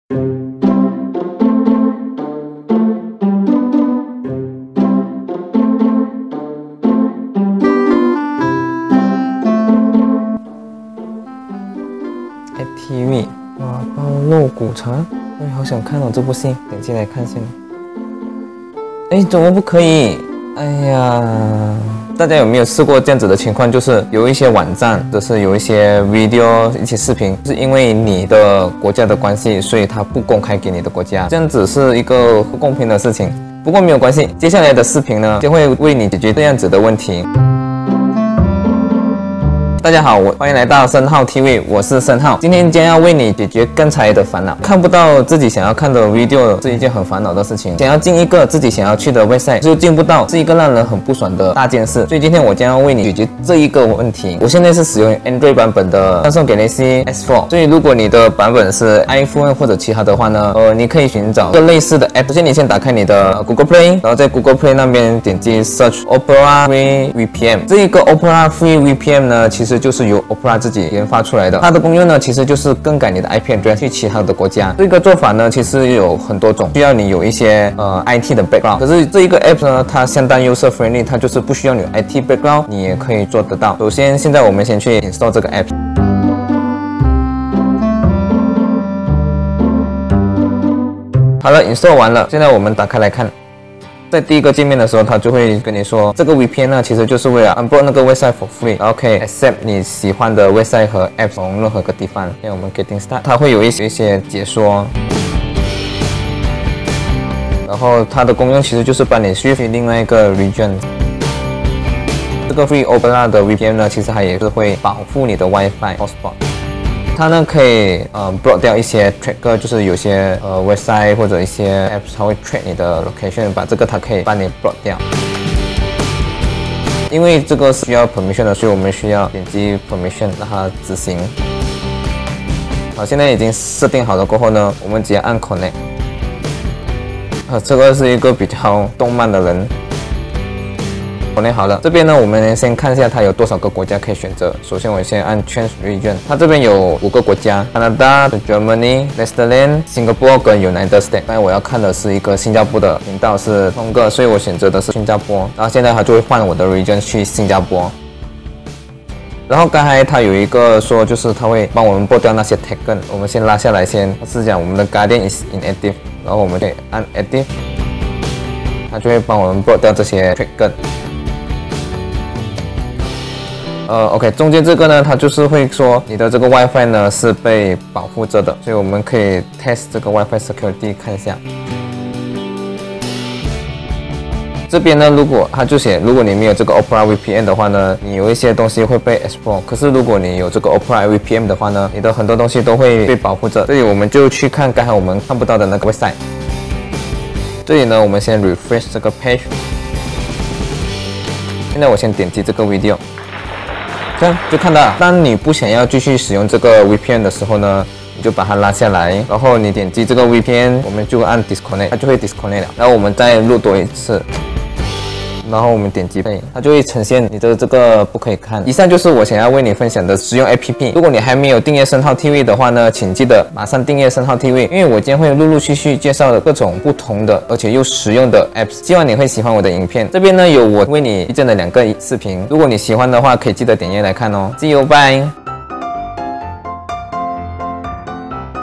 如何翻墻/访问/查看被封鎖的网站/影片?[App Review]How to access blocked websites/video?-Opera Free VPN[手機App推薦]03
今天我將爲您推薦一個手機翻墻、突破網絡封鎖的實用App.
Today I would like to  recommend a mobile phone app to let you able to view blocked website. (or how to unblock a blocked website)

00:22
大家有沒有試過這樣的情況？就是，有些網站，或是影片視頻，因爲您國家的關係，他不公開給您看。這是個不公平的事情。不過沒有關係！接下來的視頻將會爲您決解這樣的事情！

00:37
大家好！歡迎來到 SernHao Tv。我是Sern Hao.今天將要為您決解剛才的煩惱！看不到自己想要看的影片，是一件很煩惱的事情。想要進一個自己想要進的網頁，又進不到，是個讓人很不爽的，大件事！

00:54
我是使用安卓版本的三星galaxy s4。如果您的版本是IPhone(IOS), 或是其他的話呢，您還是可以找到類似的應用程式（app）。首先，您先打開您的google play，在google play哪裏點擊search：opera free vpn.

01:07
這一個Opera Free VPN,就是由Opera（公司）自己研發出來的。他的功用其實就是更改您的ip address，去其他的國家。

01:32
現在我們到開來看。在第一個界面的時候，它就會跟您說：這個VPN其實就是unblock website for free （免费解除封锁网站）。它能是幫你switch（轉換）去另一個region（地区）。這個Opera free VPN，其實他會保護您的wifi、hostspot。他可以break（阻止）一些tracker （網路跟踪器），就像一些網頁和app會追蹤您的location（地點）。這個它可以幫你block掉。

02:12
這邊我們先看它有（提供）幾個國家可以選擇。首先，我們先按“change region”。它這邊有5個國家。加拿大、德國、荷兰、新加坡、美國

02:39
然後它有說它會為我們block掉那些tracker。我們先拉下來。它說我們的Guardian（监护人） 是 inactive（待用）。然後我們按active，他就會幫我們block掉這些tracker。

02:53
按中間這個呢，它說您的wifi是被保護著的。我們可以test這個wifi security來看一下。這邊，它就寫：如果您沒有這Opera VPN 的話呢，您有些東西（資料）會被explore（搜索到）。要是您有這個 Opera Free VPN 的話呢，您的很多東西（資料）會被保護著。

03:25
當你不想再用這個VPN的時候呢，就把它拉下來。然後點擊這個vpn (Opera Free VPN)，就按disconnect（断开）。它就會disconnect了。然後我們在load（這個網頁）多一次。然後我們點擊play。它就會呈現您不可以看。

03:38
以上就是我想爲您分享的實用app。如果您還沒有訂閲SernHao Tv的話呢，請記得馬上訂閲SernHao Tv。因爲我將會陸陸續續的介紹各種不同的而且又實用的app。希望您會喜歡我的影片！這邊有我爲您推薦的兩個視頻，如果您喜歡的話，可以點擊來看哦。see you! Bye! 再見！

03:55

00:22
Do you have this kind of experience before? That is, some sites, video or film, because of your country, it does not openly show you. This is not a fair thing. But, it is ok now! In this video, I will share for you about how to solve such a thing!

00:37
Hello everyone! Welcome to SernHao Tv. I Sern Hao. Today I will be here to help you solve the troubles! Do not able to watch the video we want to watch, it is a very troubling thing. When want go to a web page we want to enter, but fail. It is very unhappy, big deal!

00:51
So, today I'm going to help to solve this problem.

00:54
I am using the Android version and Samsung galaxy s4. If your version is IPhone (IOS), or other of it, you can also find similar application (app).

01:07
The Opera Free VPN, which is developed by the Opera (companies). It is use to switch your current ip address location, to other countries.

01:32
Now we start to look at it. When the first screen, and it will tell you:The VPN is actually unblock website for free. He can access (link to) your favorite website and app, anywhere. getting start (let's begin ...) It will be some explanation. Then, his utility is to help you switch to another region. The Opera free VPN, in fact, he will protect your wifi or hostspot. He could break (prevent) some of the tracker (web tracker), Like some pages and the app keeps track of your location. This can help you block it out. Because of this, it need permission, So we need to click the permission, let him perform.

02:12
Here we look at it can provided in several countries to select with. First, we press the "change region". It is here five countries. Canada, Germany, the Netherlands, Singapore, United States.

02:39
Then it said it would help us to block those tracker. Let's scroll it down. It is said that our Guardian is inactive now. Then we press active, He will help us block out these tracker.

02:53
Press the middle tab, it says your wifi is  protected. We can test this wifi security. Here, it is written: If you do not do this, then Opera VPN, Your Something (data) will be explore (to others). If you have the Opera Free VPN, many of your stuff (data) will be protected.

03:25
When you do not want to use it (Opera Free VPN), pull it down. Then click on the vpn (Opera Free VPN), Click "disconnect". It will disconnect then.

03:38
That is all I want to share the useful mobile app for you. It is because I will continue to introduce a lot of useful app. Here, I recommend 2 videos for you, If you like it, you can click on to watch. see you then! Bye ~!

03:55